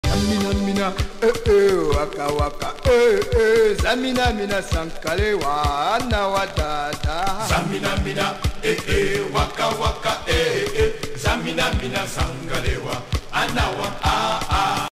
BAM - Spaniel na wysokości 3 m
Bam Cocker Spaniel Angielski na drabinie i kładce o  wysokości 3 m.
Trenujemy w Ośrodku Szkolenia Psów Kamiron  W Częstochowie